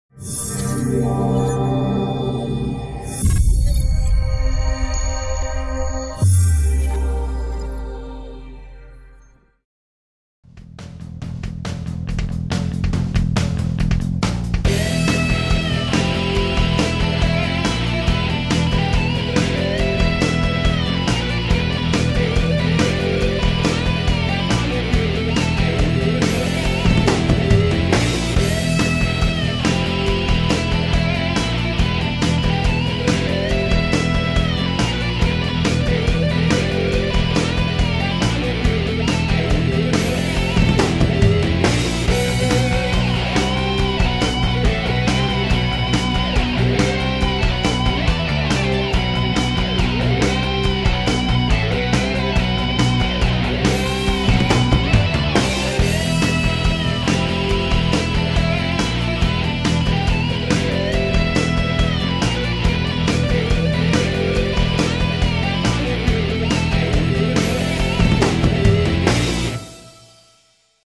EVAAB Terry Orr Ford Ranger 2017 Short
Terry Orr converted a Ford Ranger to an EV.